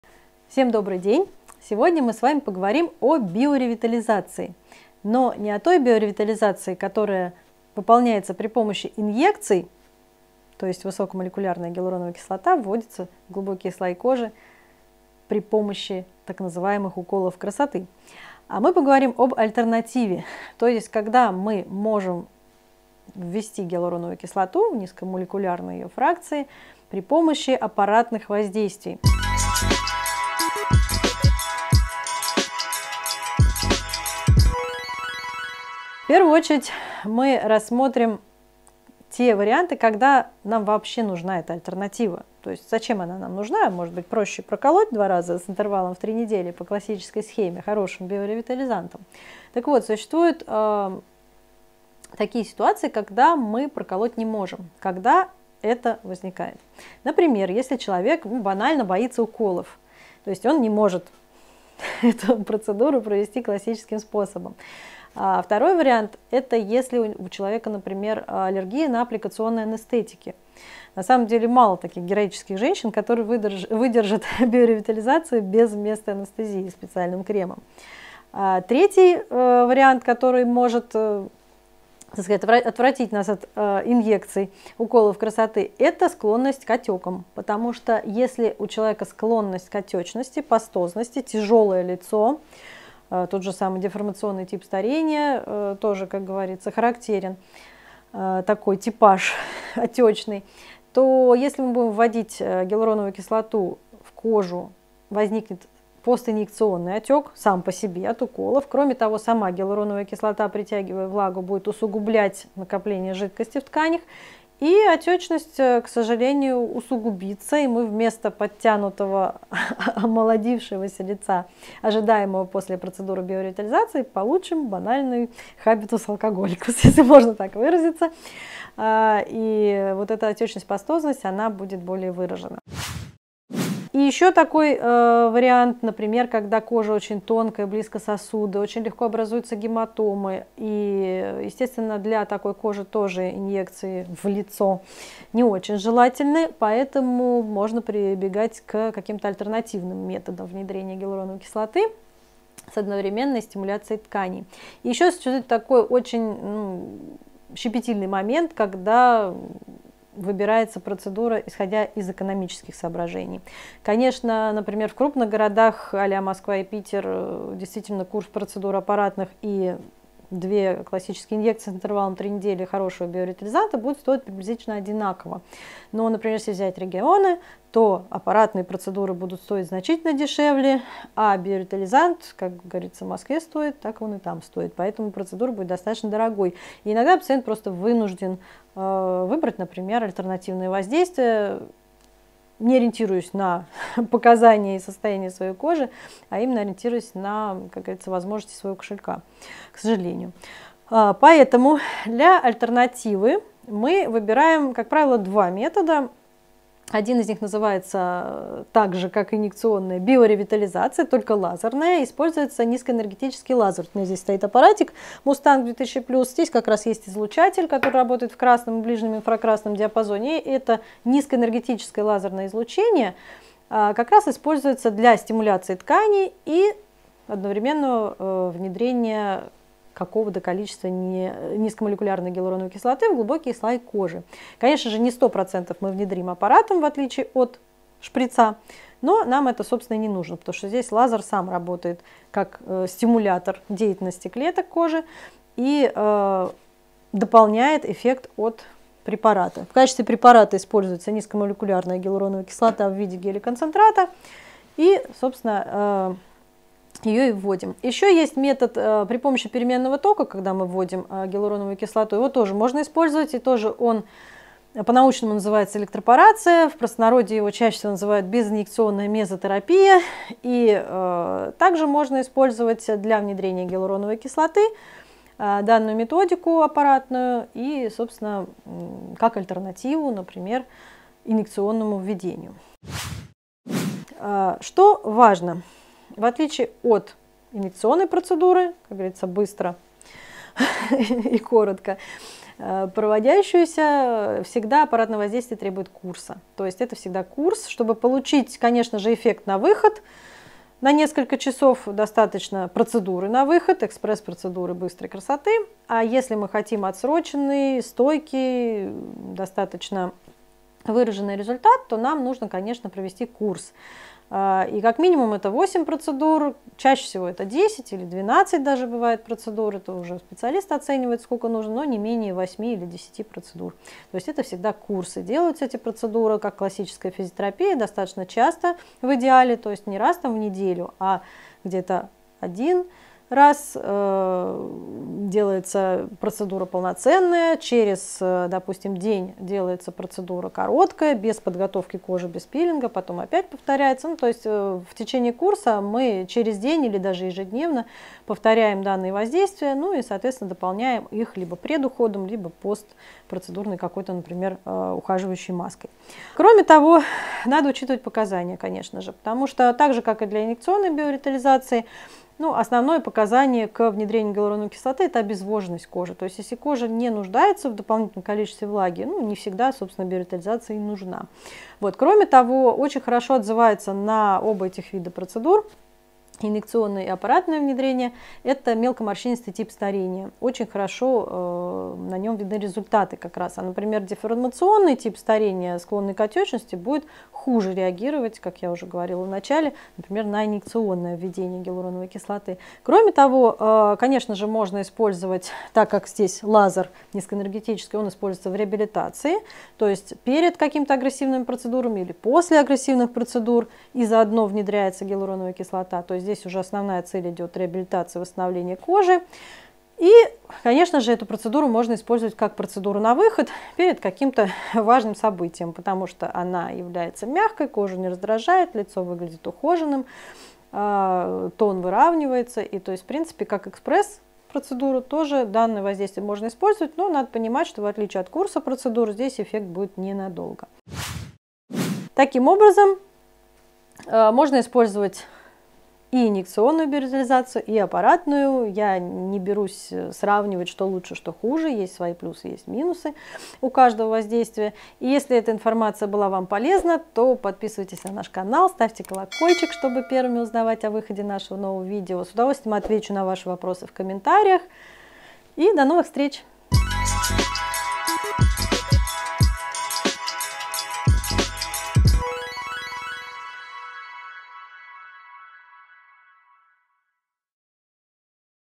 Биоревитализация лица / Альтернативный способ без уколов
Биоревитализация. Альтернативный способ.
В данном видео мы поговорим о том, чем заменить инъекционную биоревитализацию, если для вас, по какой-то причине, нежелательны «уколы красоты». Например, вы боитесь уколов или имеете аллергию на аппликационные анестетики, у вас хрупкие сосуды или склонность к отёкам. Мы обсудим, какими методами, кроме шприца, можно ввести гиалуроновую кислоту в глубокие слои кожи, разберем, какие особенности и показания есть у подобных процедур.

НОВИНКИ! __________________________________________
ГДЕ КУПИТЬ?
Выбирайте бесконтактную доставку.
Не болейте!